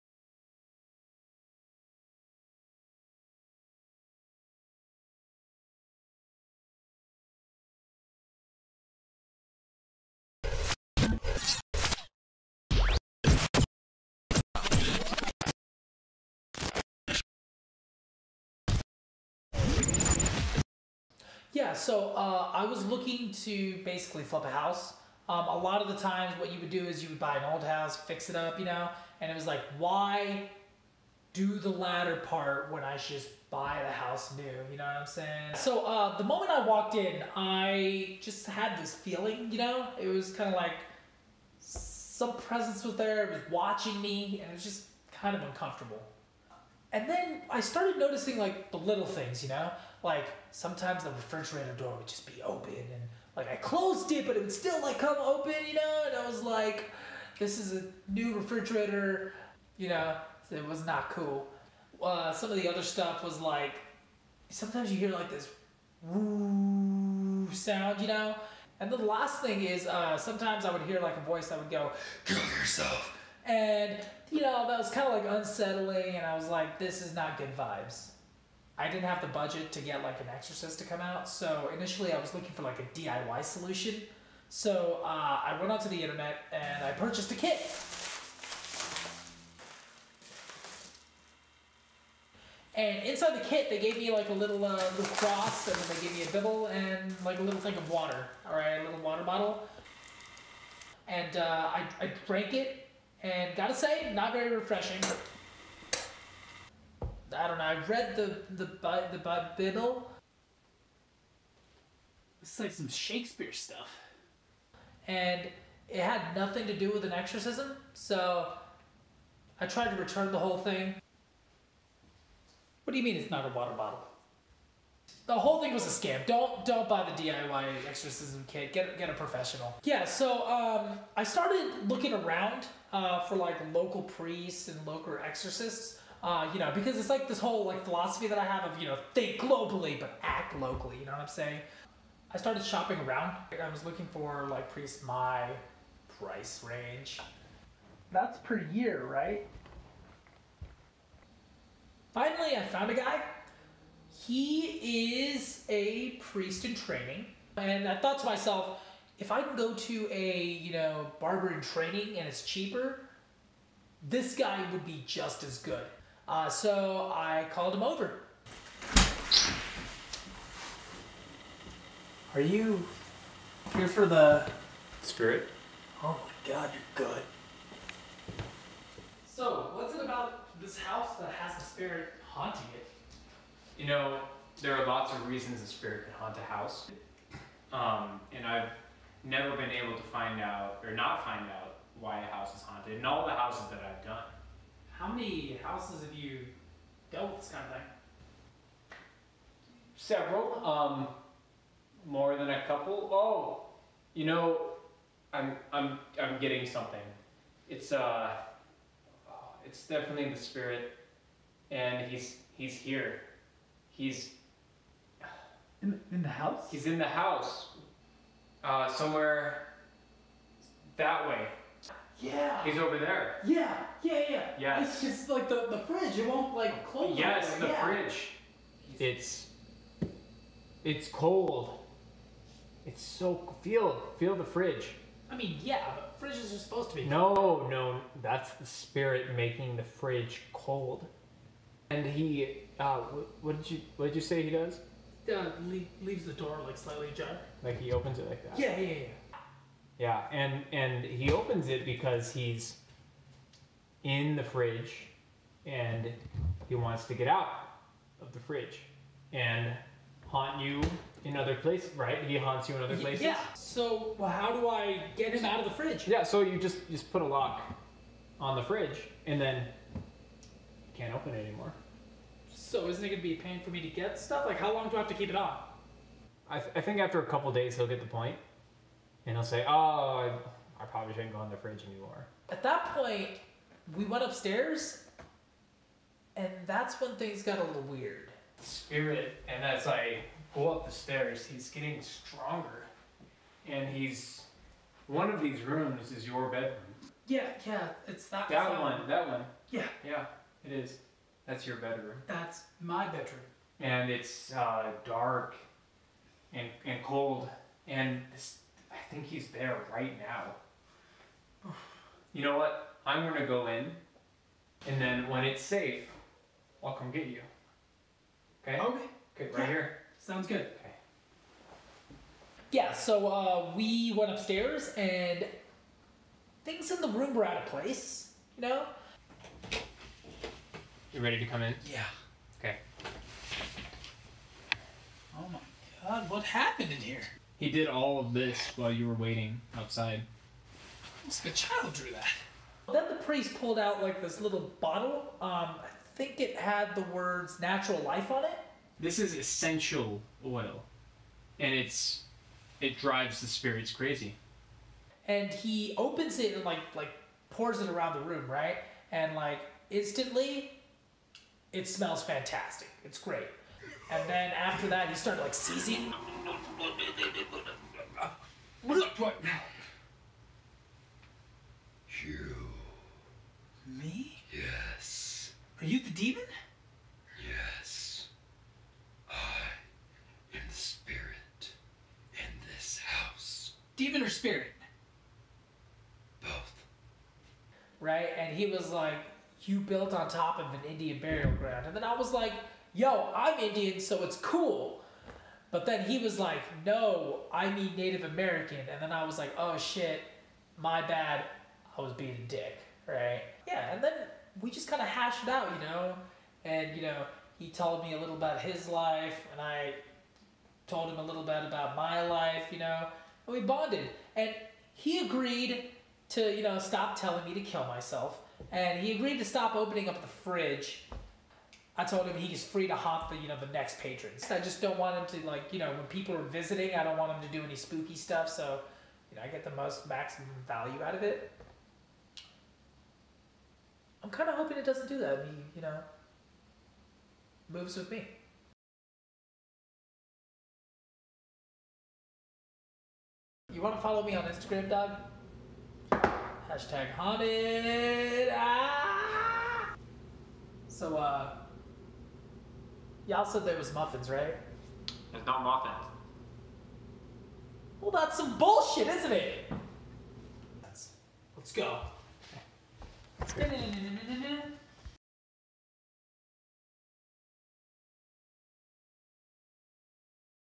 My Haunting Story
A terrifying (true?) story about a man and his ghost.